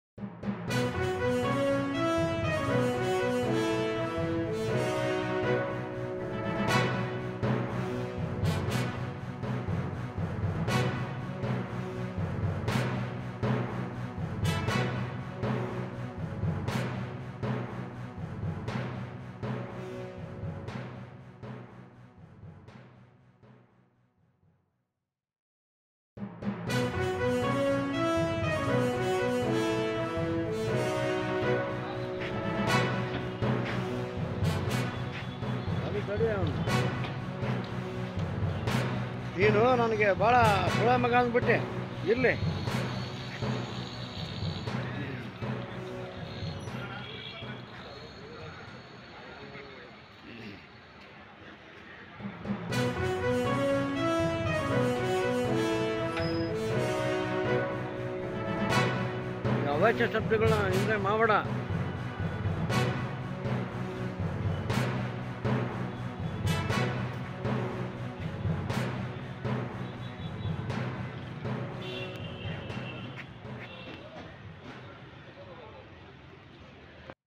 Challakere suspended PDO who was driving around drunk : ಚಳ್ಳಕೆರೆ ಕುಡಿದು ತೂರಾಡುತ್ತಿದ್ದ ಪಿಡಿಒ ಅಮಾನತ್ತು
Karnataka | ಕರ್ನಾಟಕ
Kannada | ಕನ್ನಡ